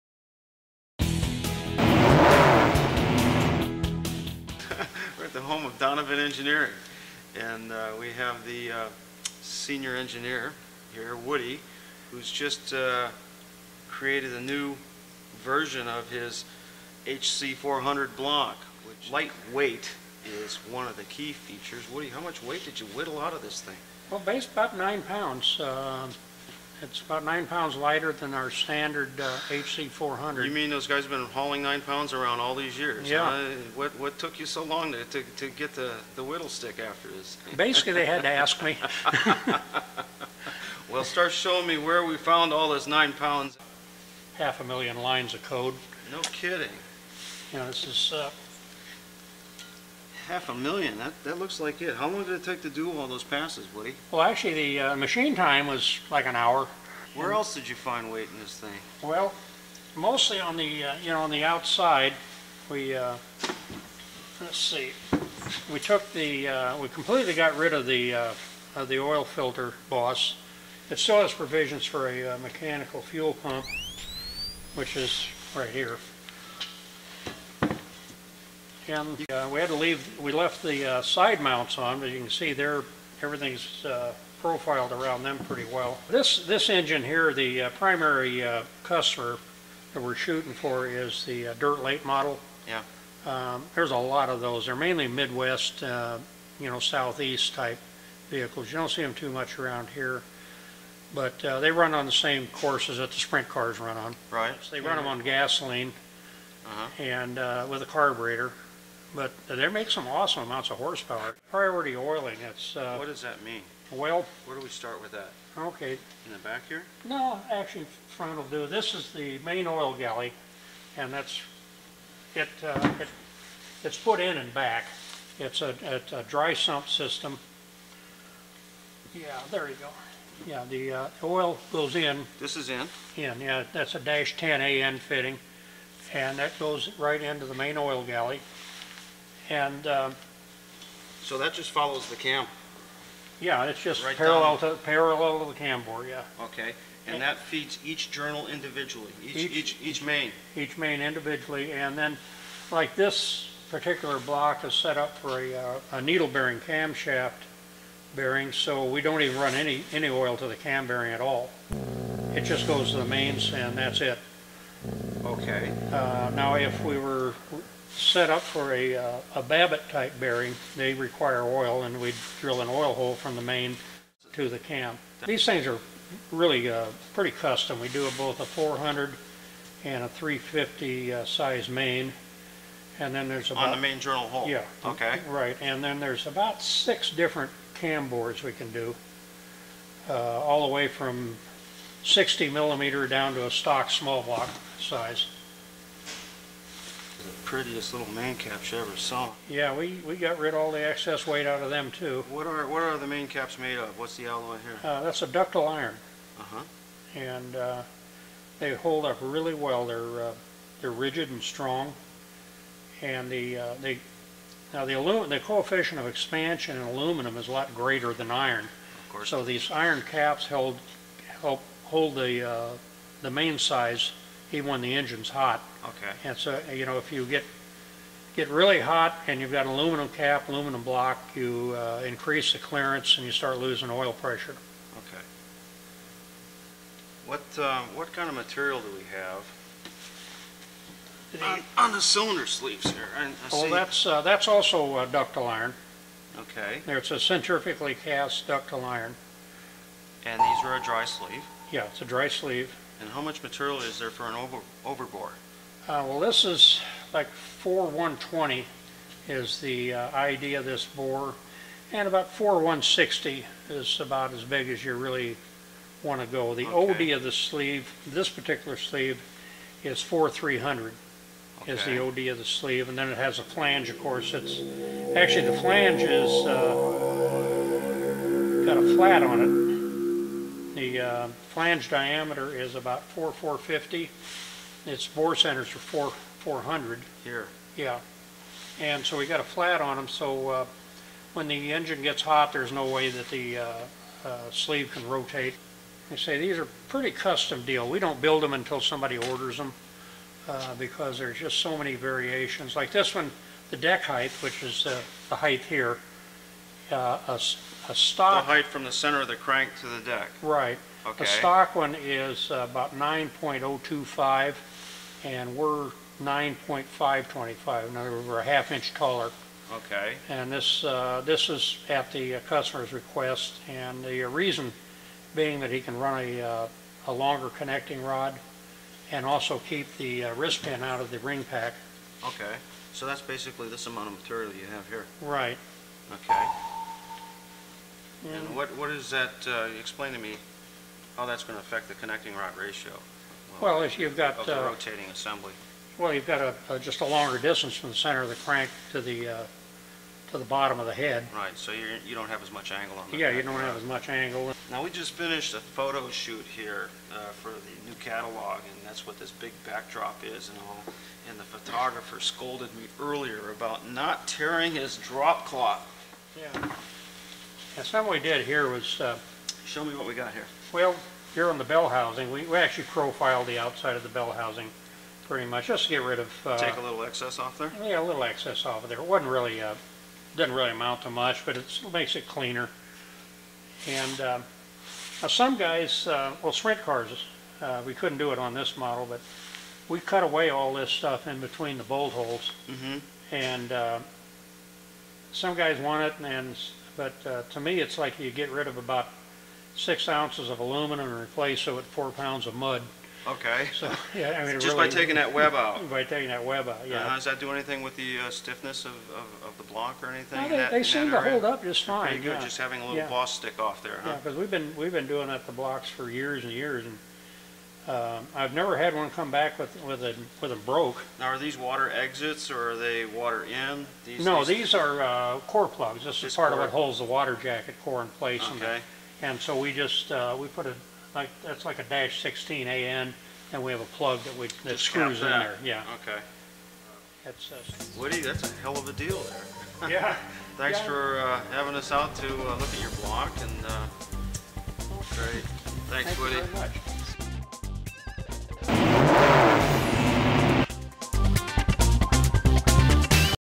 Drag Racing's Woody talks about his HC400 Lightweight Donovan
After a product shoot at Donovan Engineering, "Lister Bob" Santoro got Norman "Woody" Woodruff to chat about some amazing features of his new Lightweight HC400 block.

Those who grew up in the musclecar era already know about these coveted Donovan blocks.  In case you missed it, this clip offers a little insight into the design philosophy that led this mill into the winners circle more times than not.

Bob growls in disbelief when Woody reveals the oiling secrets of needle cam bearing design.

Priority oiling is covered along with the advantage running of a tall deck.  Beautiful close ups of the CNC millwork is shown in great detail.  Woody even discloses the reasoning behind the clever sleeve flange design that keeps things in place.

It's a Zen-like revelation, and even Woody could hear the monks chant...though he tries to ignore it.